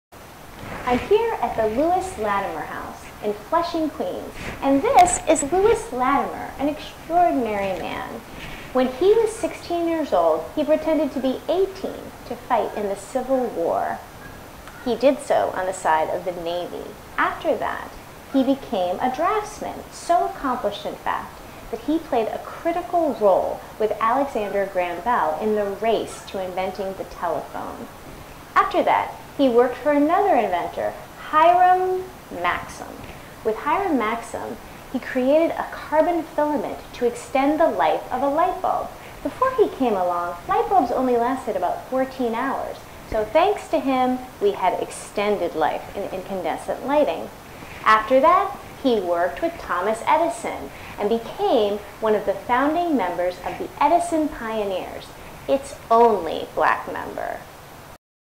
Lewis Latimer House
Lewis Latimer made history many times. First, Alexander Graham Bell won the telephone race thanks to Latimer's crucial help. A hero of invention and ingenuity, Lewis went on to make lightbulbs much better. He invented their crucial carbon filament. In fact, before Latimer's filament patent, light bulbs only lasted about 14 hours. Thanks to his carbon filament, their illumination thus endured much longer.

Later in his career, Thomas Edison honored him as a founding member of the Edison Pioneers. An extraordinary genius, Lewis Latimer did all this with no formal education. The child of famous fugitive slaves, his youth was no picnic. In truth, he was notorious even before birth. Visit the Lewis Latimer House Museum in Flushing, Queens for more incredible accomplishments and insights into this extraordinary history-maker.

Learn more about this and other incredible stories from history with a visit to LadyKflo's site: